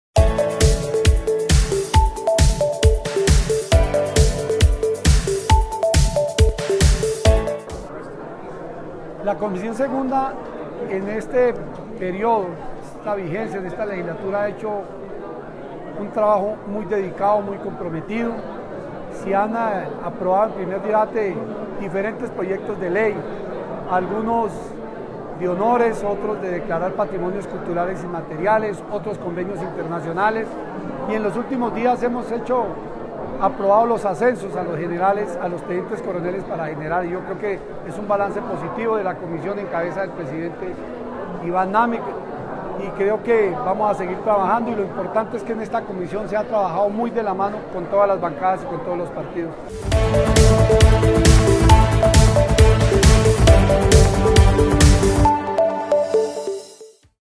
Balance Legislativo Comisión Segunda / 20 de julio a 16 de diciembre de 2017
Balance Legislativo / 20 de julio a 16 de diciembre de 2017

Presidente: Iván Name Vásquez
Vicepresidente: Rigoberto Barón